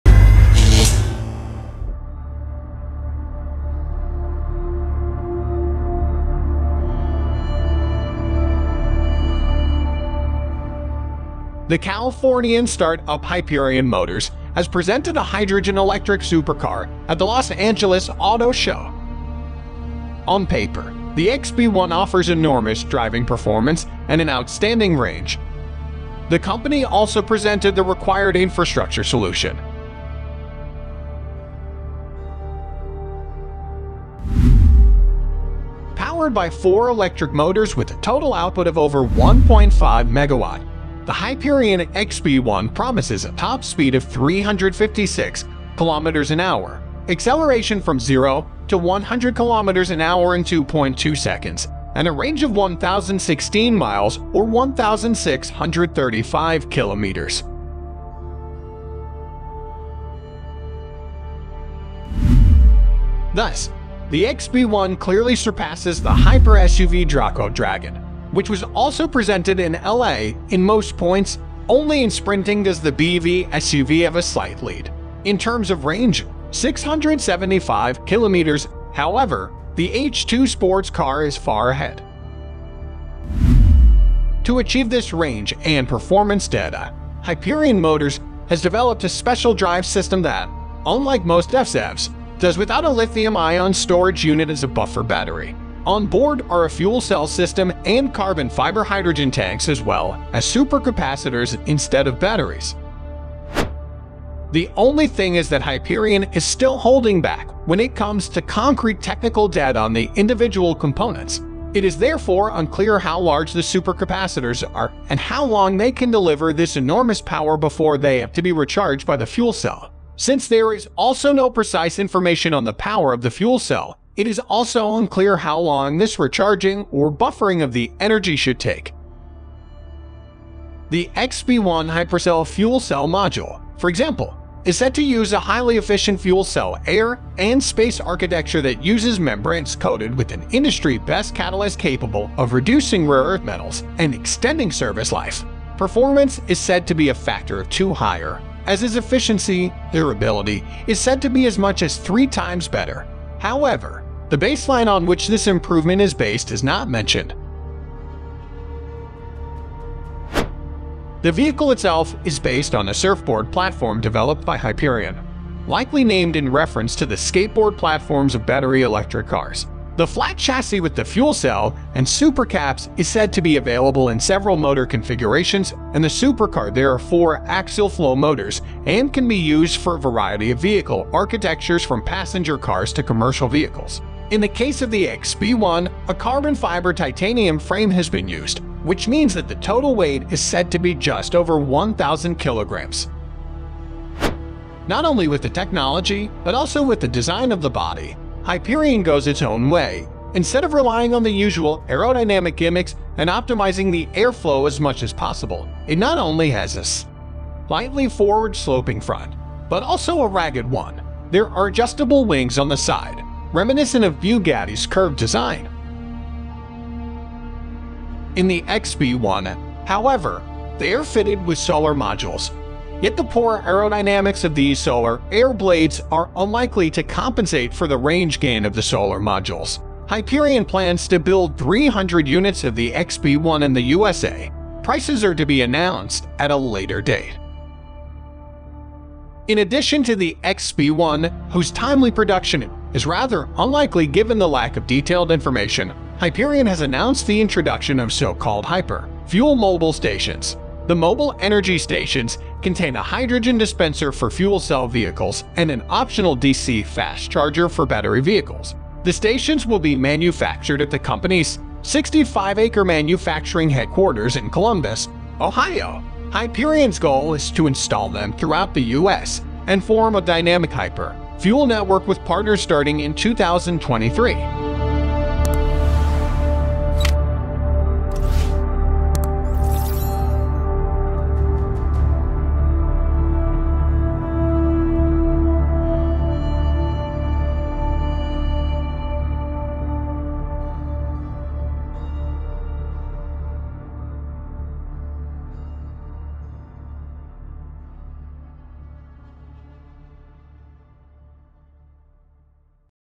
Toward the Future: Unleashing the Power of Hydrogen with Hyperion XP-1
The Hyperion XP-1 is a groundbreaking marvel in the automotive world, epitomizing a visionary future of sustainable transportation. This cutting-edge supercar draws its inspiration from hydrogen technology and is designed to deliver high-performance capabilities without harmful emissions.

With an innovative hydrogen fuel system, the Hyperion XP-1 offers an extraordinary driving experience, boasting rapid acceleration and astonishing top speeds. Meticulously selected carbon-based materials for the car's body provide both strength and lightweight, enabling the vehicle to accelerate with unmatched speed and maneuver with utmost agility.